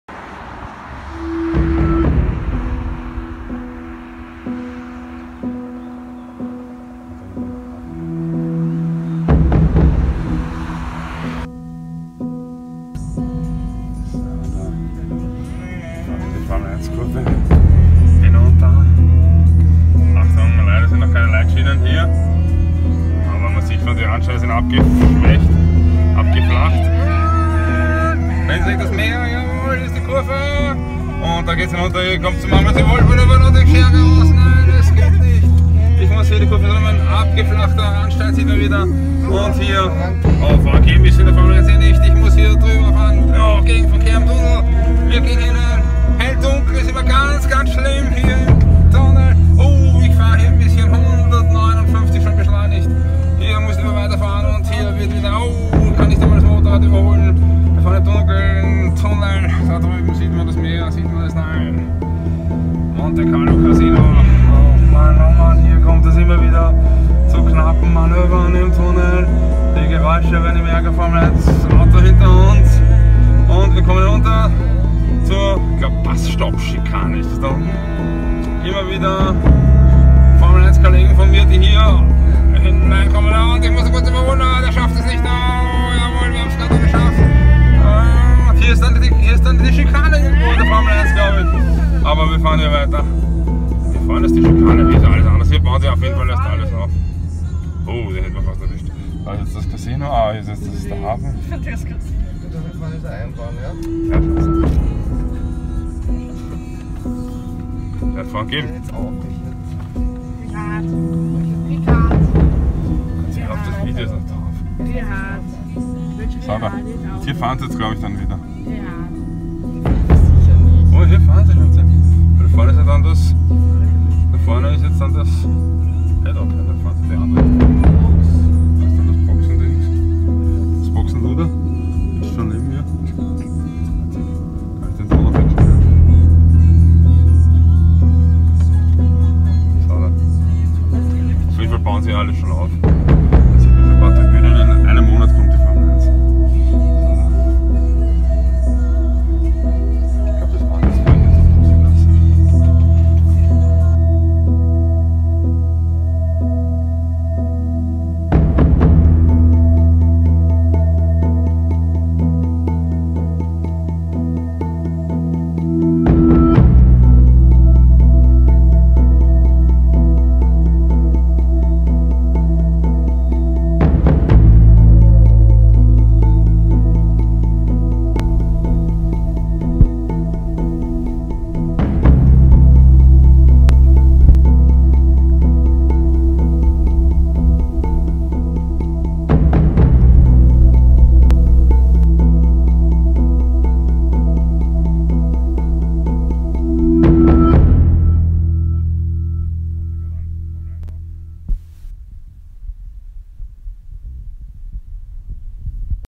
Monaco F1 GP Kurs - Monte Carlo
Fahrt über die Formel 1 Strecke 2018 in Monaco
Alle berühmten Kurven, Geraden und Tunnel hier mit Family...
Im Mai startet das Rennen - schon jetzt beginnt der Auf- und Umbau - 6 Wochen Aufbau , nur 3 Wochen der Rückbau
Sainte-Dévote, Mirabeau, Loews bzw. Fairmont usw
Tolle Stadt - tolle Boote und traumhaftes Wetter, wenn man sich das nur leisten könnte ;-)
Diesmal nicht mit einem Mercedes, Red Bull oder Ferrari - NEIN mit einem Bii  EM dabblJU   LOL
Circuit de Monaco